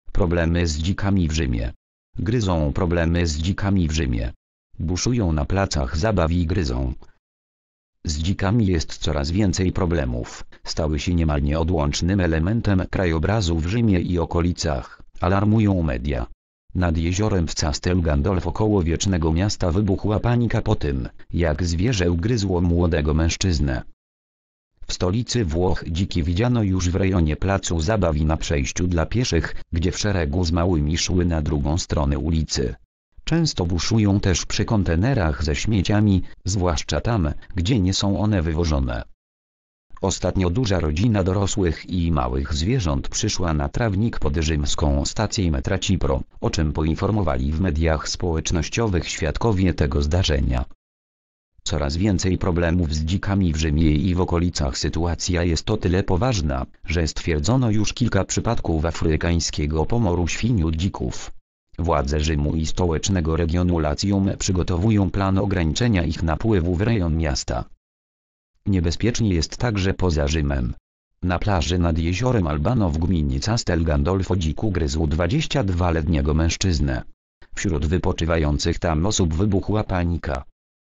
Problemy z dzikami w Rzymie. Gryzą.
Problemy z dzikami w Rzymie. "Buszują na placach zabaw i gryzą"

  (fot. AFP/ALBERTO PIZZOLI)